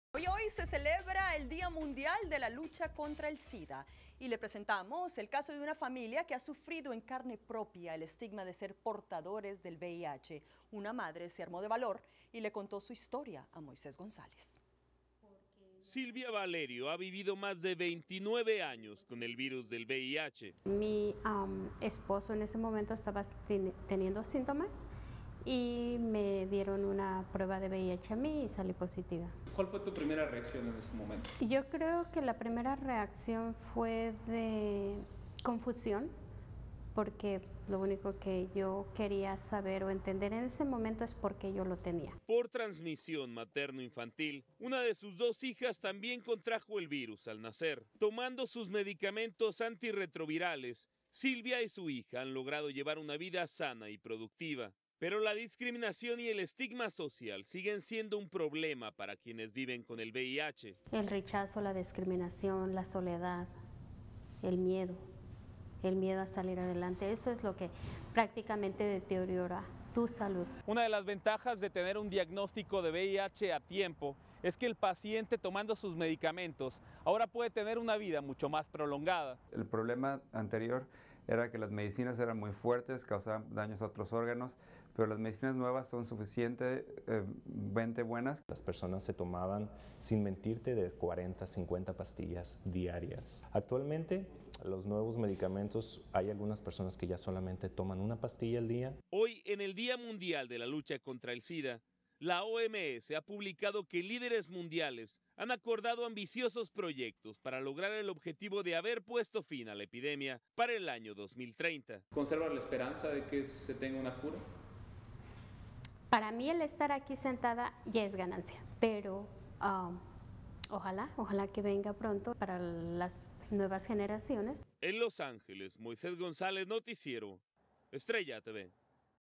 Lucha contra el SIDA.
Reportaje de Moisés González para Estrella Tv transmitido en cadena nacional en Estados Unidos.